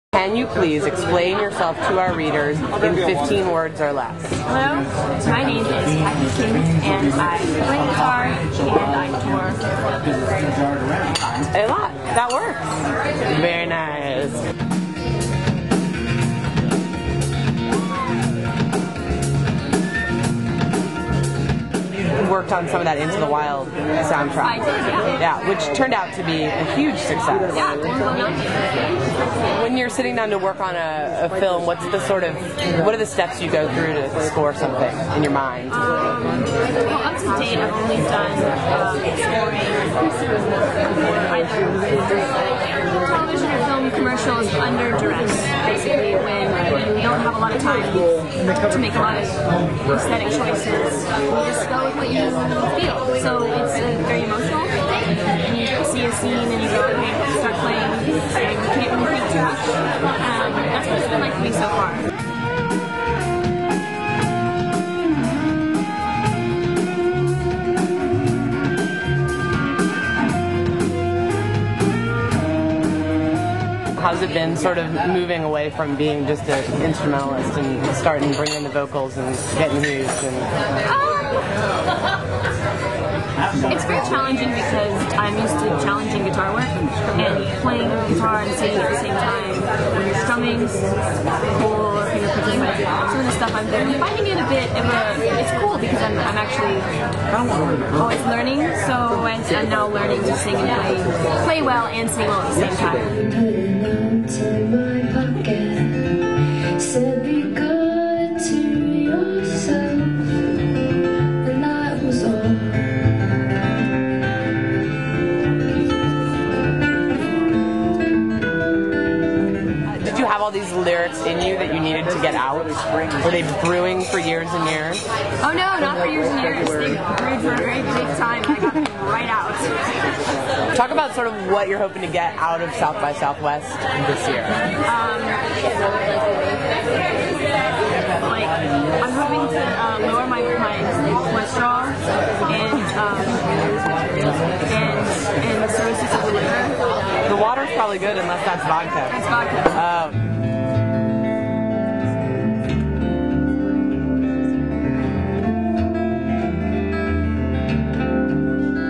Dinner With Kaki King | SXSW 2008 | Entertainment Weekly
An interview and performance from South by Southwest. By Whitney Pastorek.